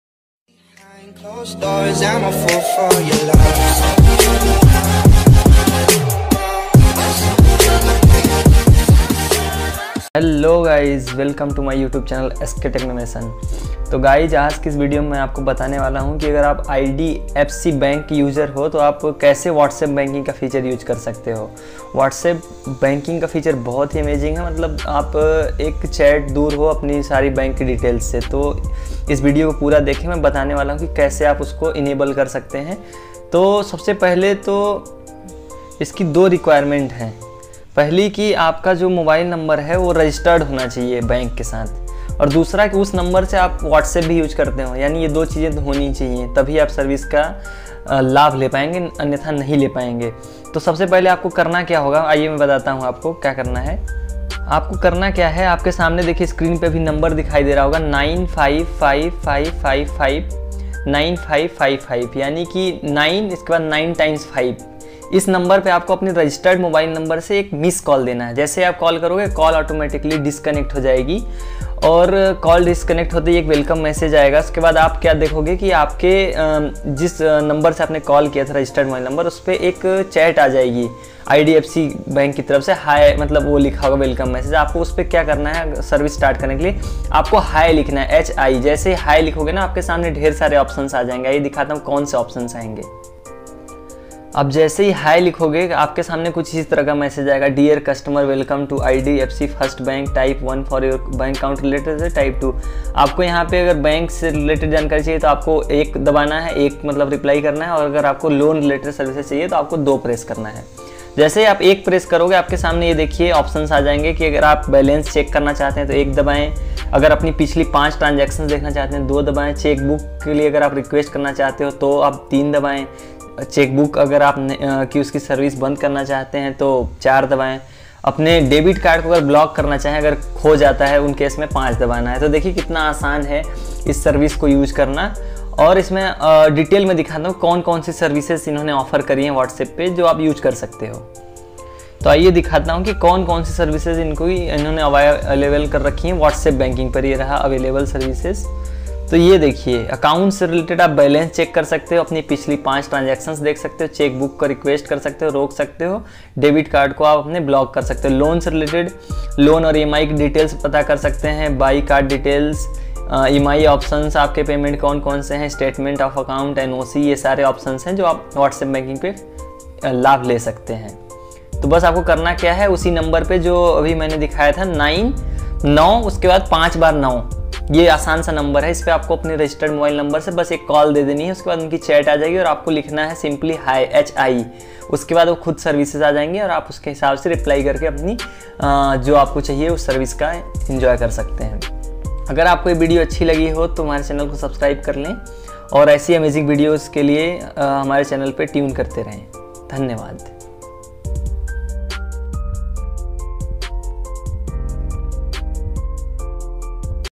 IDFC Whatsapp Banking Services 2022 || IDFC Ki whatsapp banking shuru kare Free me
This video is all about how to start whatsapp banking in IDFC , so if you are a customer of IDFC bank then this video is for you.
Is video me btaya gaya hai ki kaise ap IDFC me whatsapp banking shuru kar sakte hain.